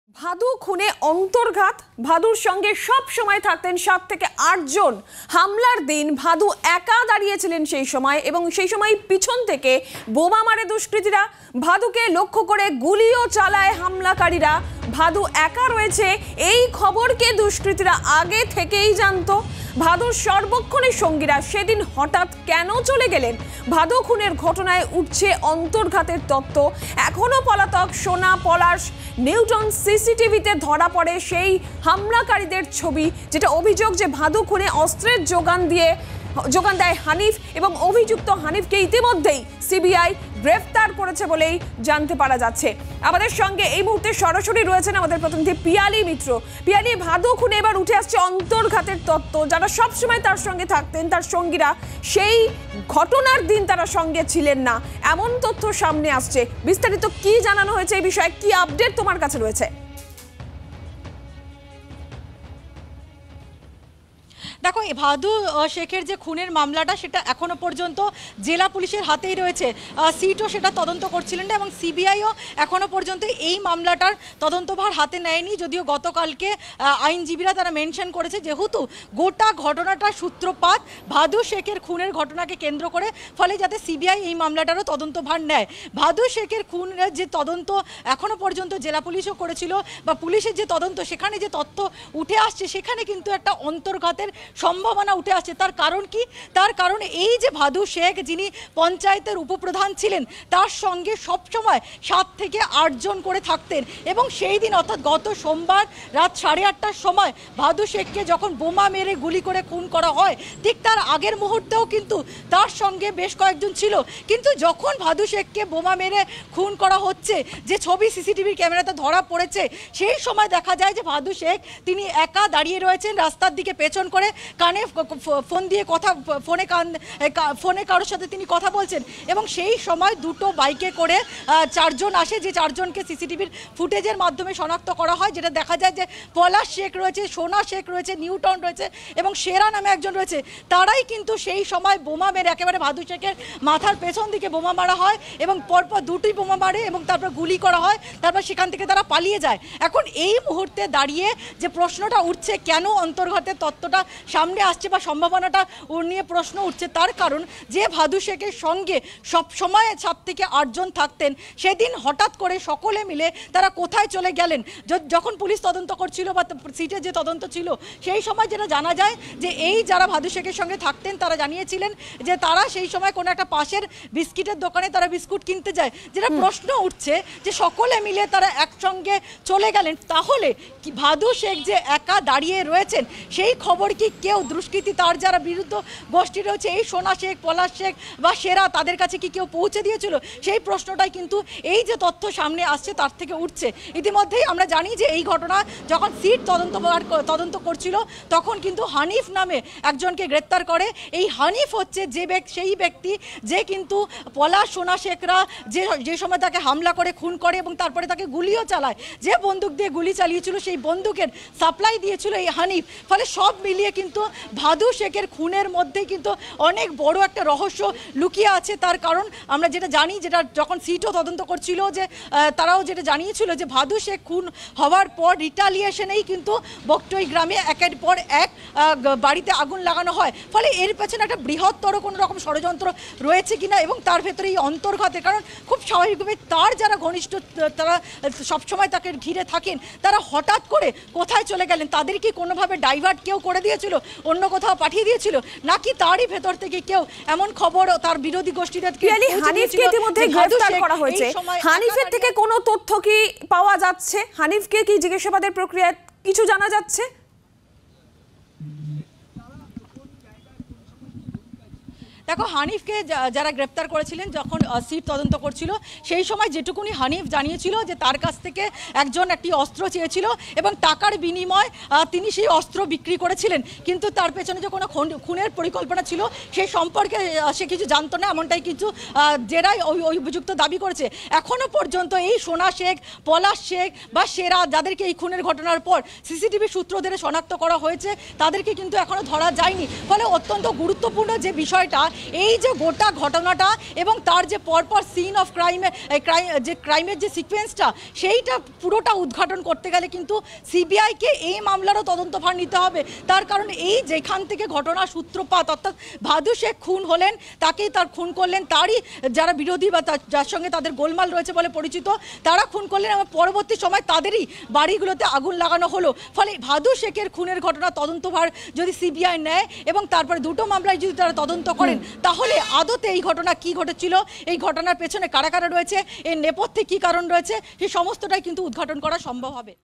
Rampurhat Arson: প্রশ্ন উঠছে ভাদু একা রয়েছে, এই খবর কি দুষ্কৃতীদের কাছে আগে থেকেই ছিল? | EXCLUSIVE
Rampurhat Arson: প্রশ্ন উঠছে ভাদু একা রয়েছে, এই খবর কি দুষ্কৃতীদের কাছে আগে থেকেই ছিল? ভাদুর সর্বক্ষণের সঙ্গীরা, সেদিন হঠাৎ কেন চলে গেলেন?